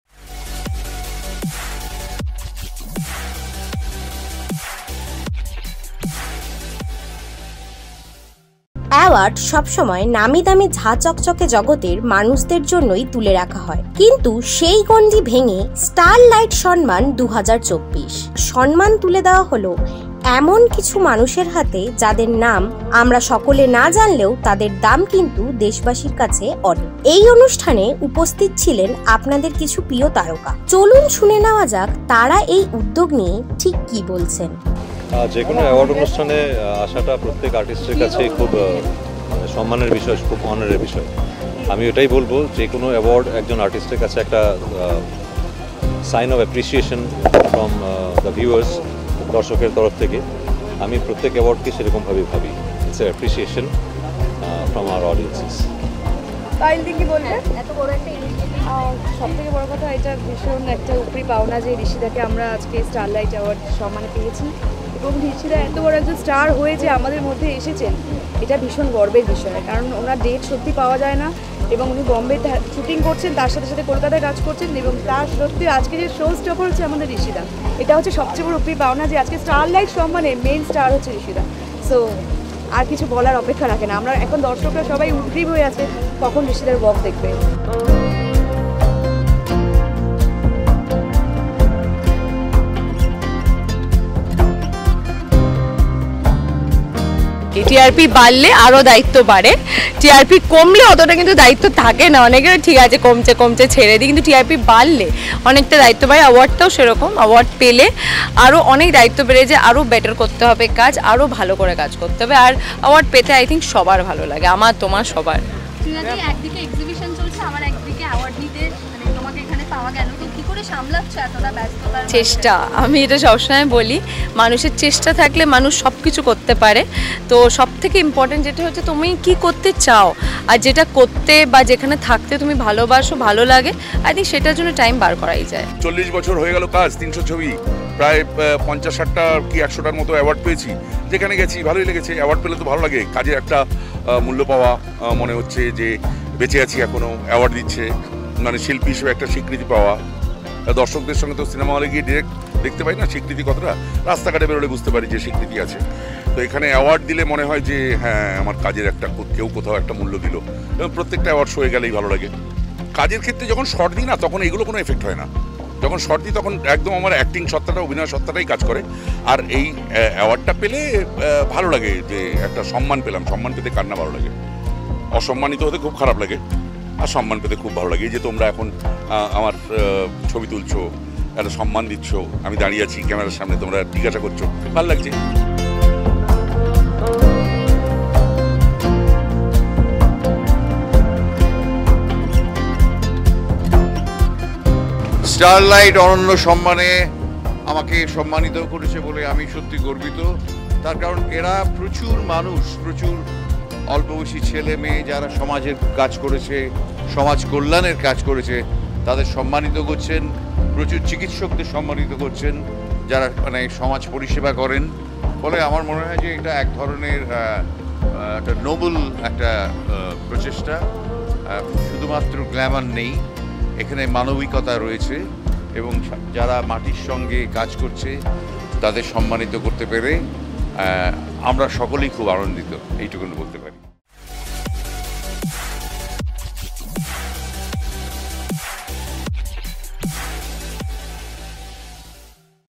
কার কার হাতে উঠলো স্টার লাইট সন্মান? Trina Saha | Rishi Kaushik | Sumit Ganguly
Star light sonman 2024, আওয়ার্ড সবসময় নামিদামি ঝা চকচকে জগতের মানুষদের জন্যই তুলে রাখা হয়, কিন্তু সেই গণ্ডি ভেঙ্গে এবার সন্মান তুলে দেওয়া হলো এমন কিছু মানুষের হাতে যাদের নাম সকলে না জানলেও তাঁদের দাম কিন্তু দেশবাসীর কাছে অনেক। এই অনুষ্ঠানে উপস্থিত ছিলেন আপনাদের প্রিয় কিছু তারকারা চলুন শুনে নেওয়া যাক তারা এই উদ্যোগ নিয়ে কি বলছেন।